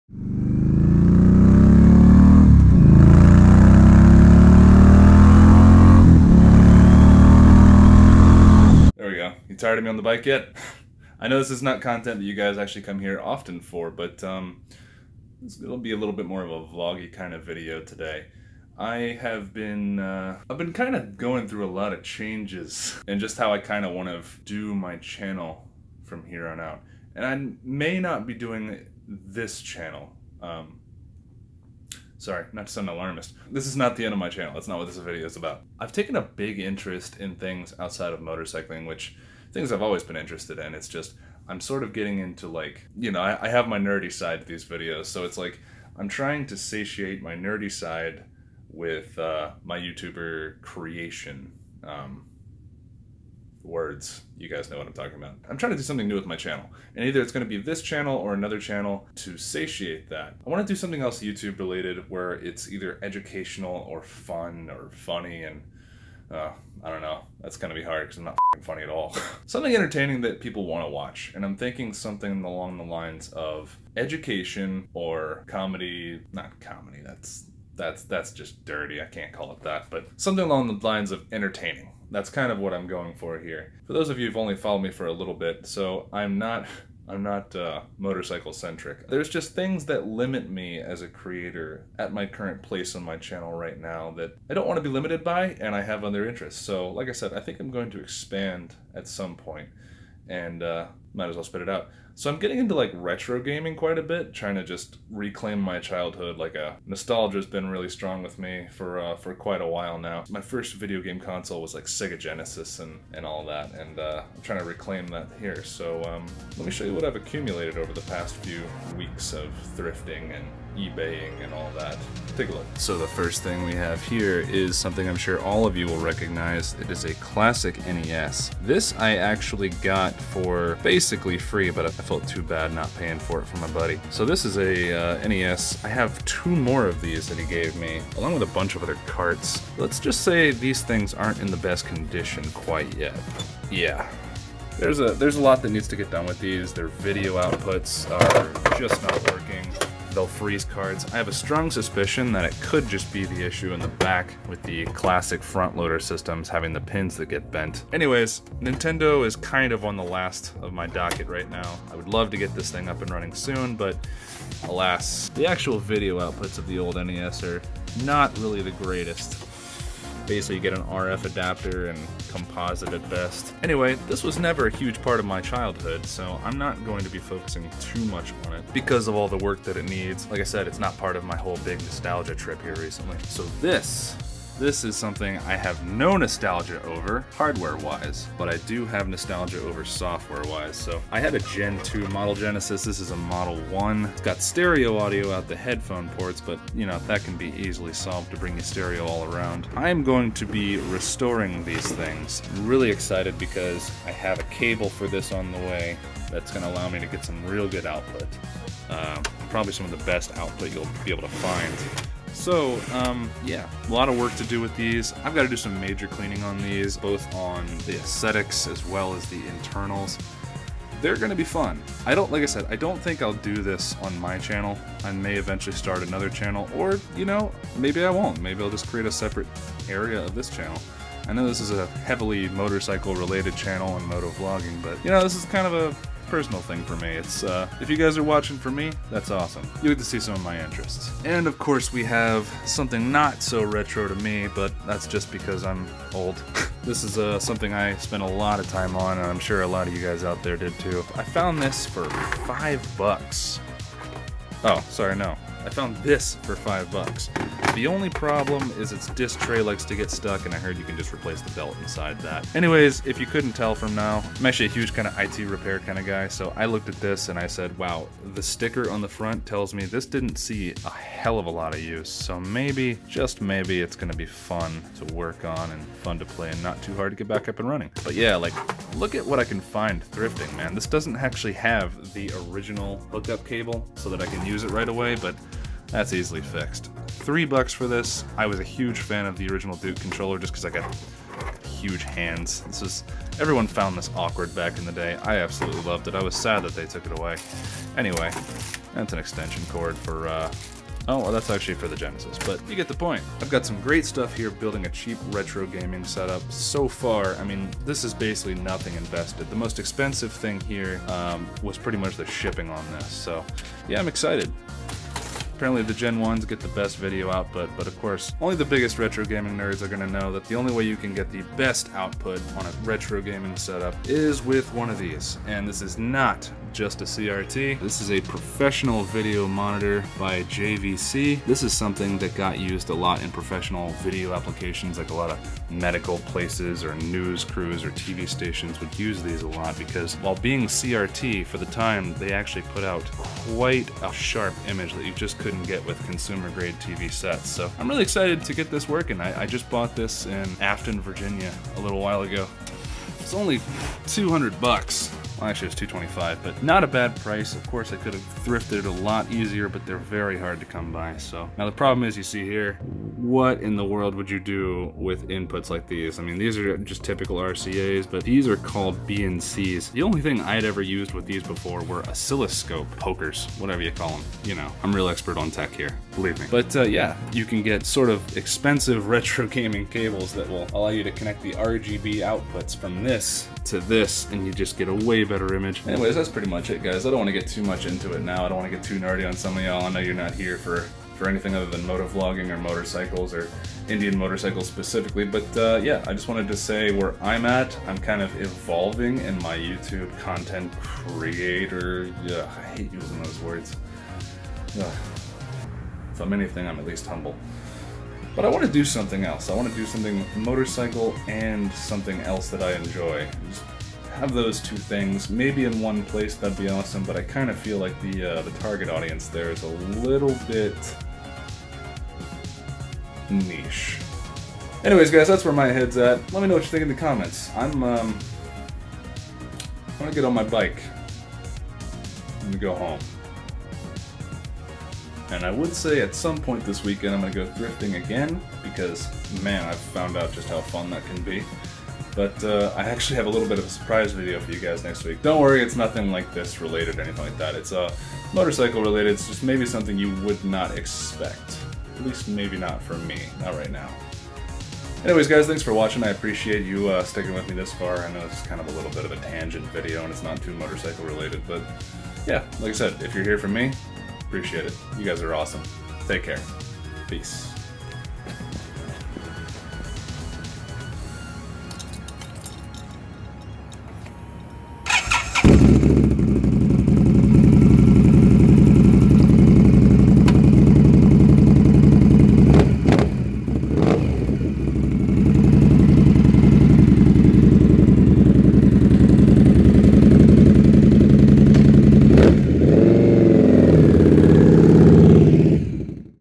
A New Direction...
Thrifting? Restoration? Gaming? Streaming? I wanna expand. Where should I take this little venture to next?